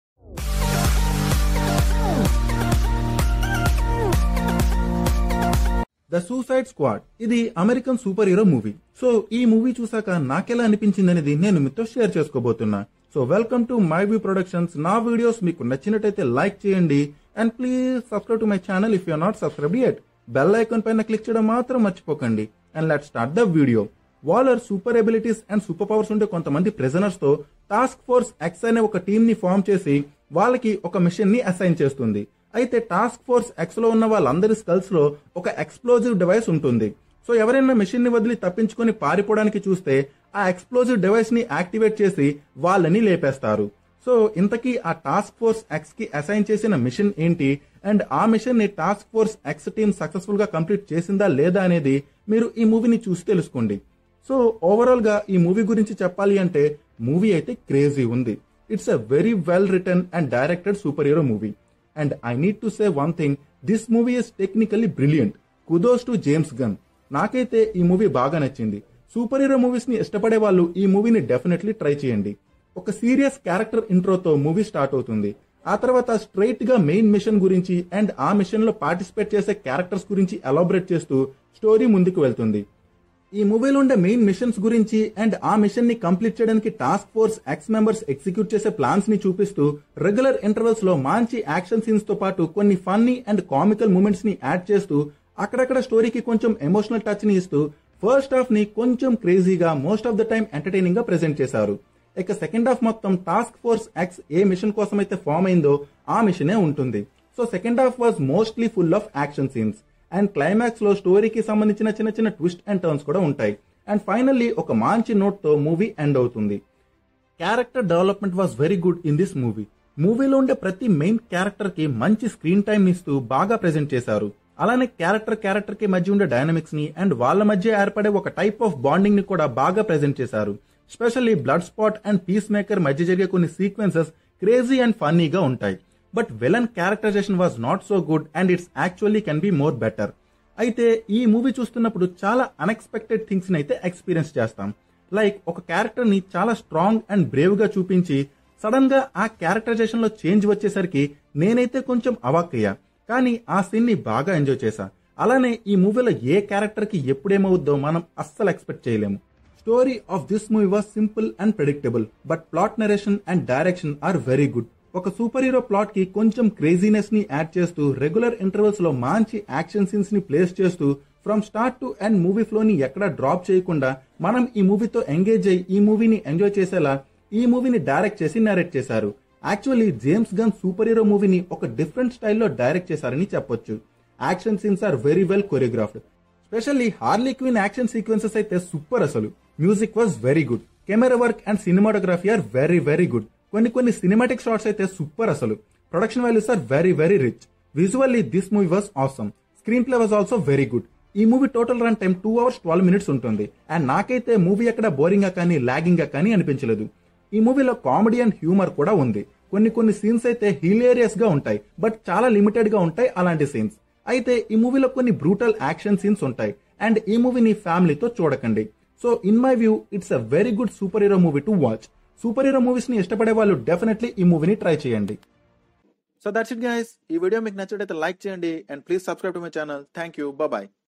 The Suicide Squad Review Telugu
The Suicide Squad is an American Fantasy Adventure Movie Review.

The Suicide Squad Review ,
The Suicide Squad Movie Review,
The Suicide Squad Review in Telugu
The Suicide Squad Telugu Movie Review,

Review Follows couple of things in the movie and mentions about good and bad aspects in the movie from My View.. What did you think of the movie ???

Instagram Username : mviewmovies